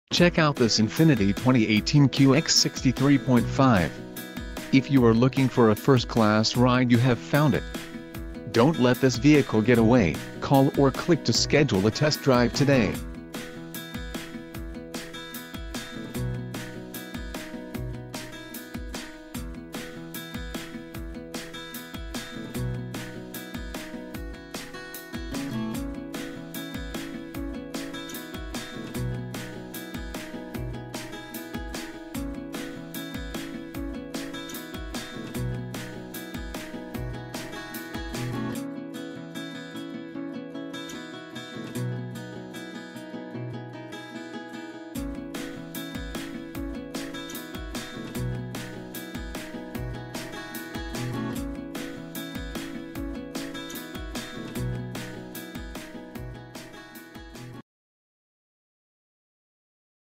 2018 Infiniti QX60 3.5 - Liberty GMC - Peoria, AZ 85382
Used 2018 INFINITI QX60 3.5, liquid platinum exterior, wheat interior, 6, 1-speed cvt w/od, 68,936 miles, Stock G11224C, VIN: 5N1DL0MN9JC504511,